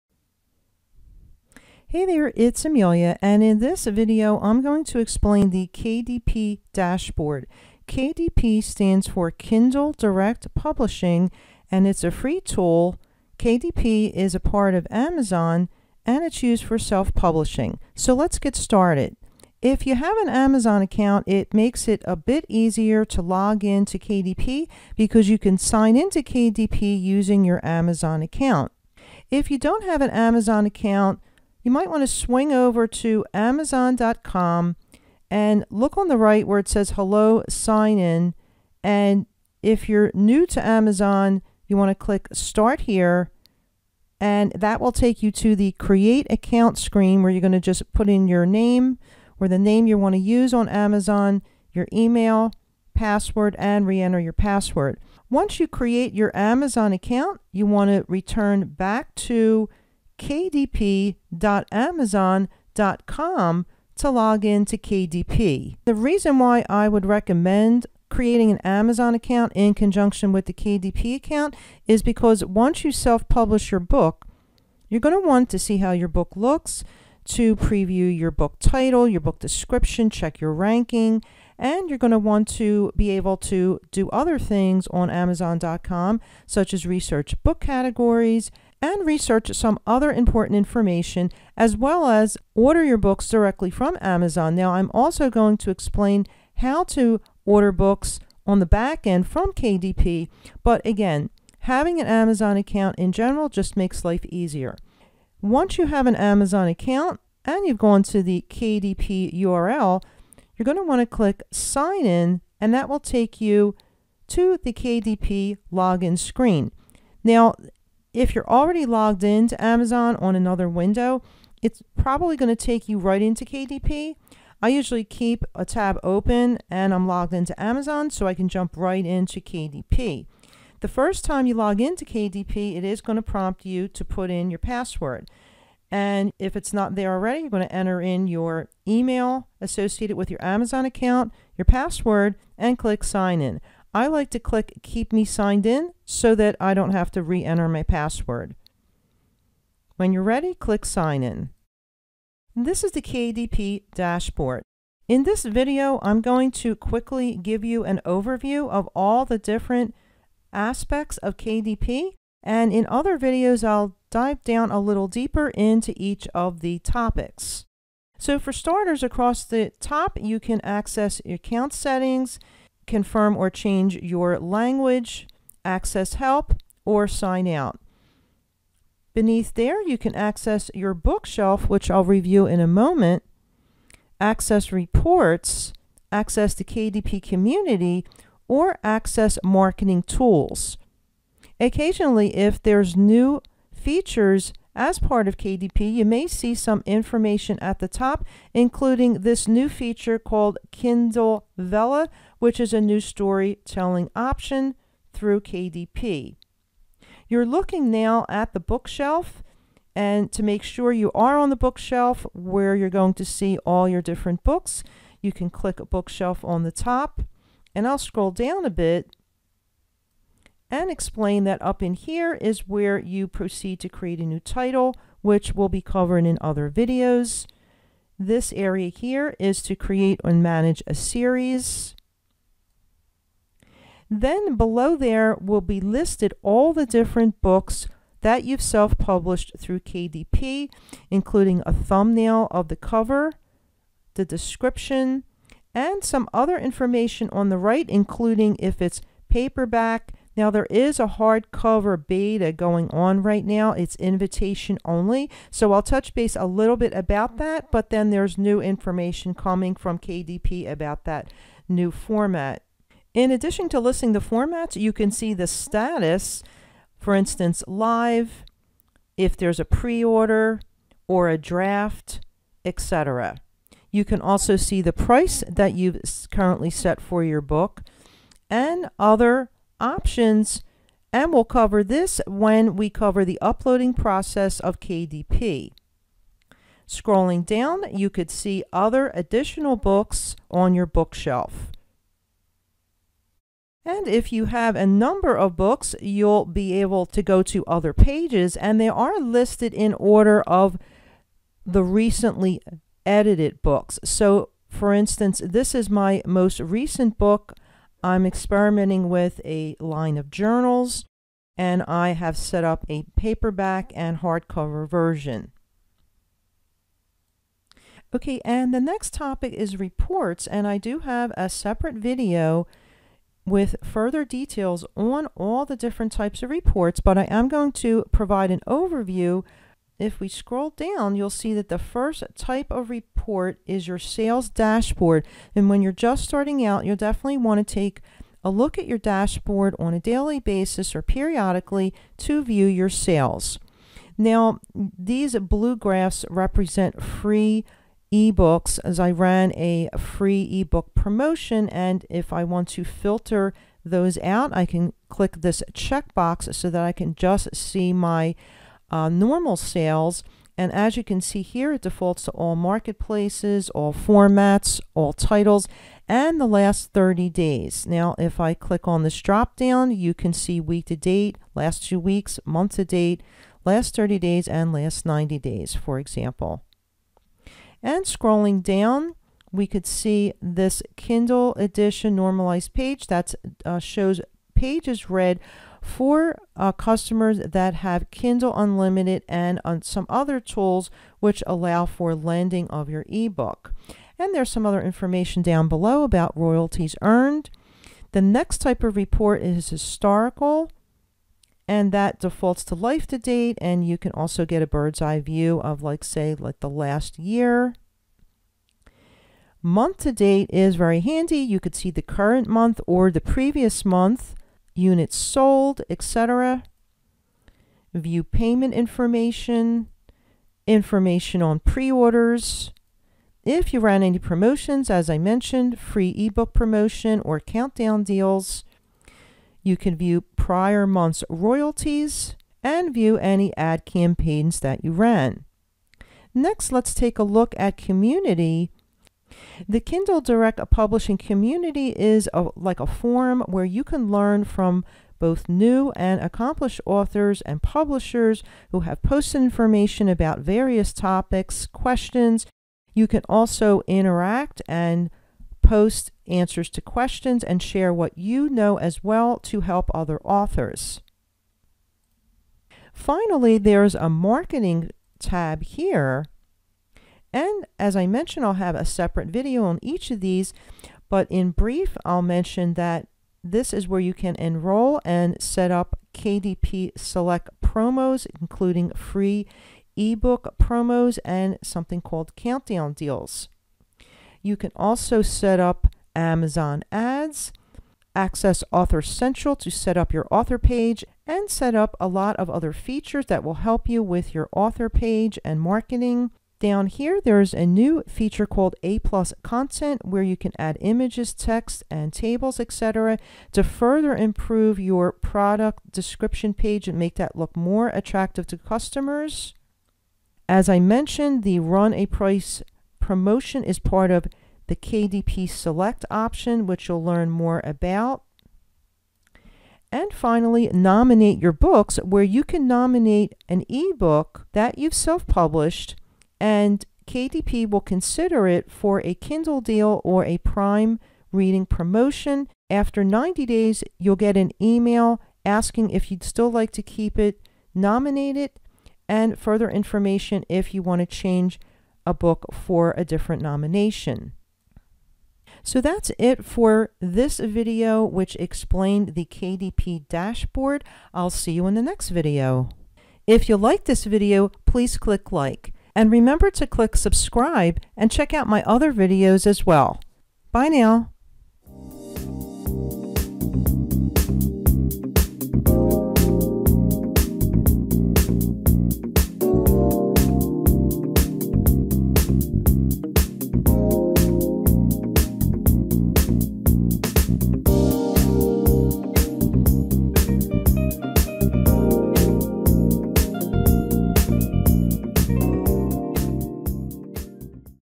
AMAZON KDP DASHBOARD OVERVIEW TUTORIAL | KDP ACCOUNT SETUP | AMAZON SELF PUBLISHING | KDP TRAINING
AMAZON KDP DASHBOARD OVERVIEW TRAINING VIDEO
00:00 - 00:18 Introduction
00:18 - 02:29 KDP Account Setup and Login
02:30 - 03:26 Dashboard
03:27 - 05:16 Bookshelf
05:17 - 07:15 Reports
07:16 - 09:04 Marketing
09:05 - 09:47 Closing

By Amelia Griggs @ 2020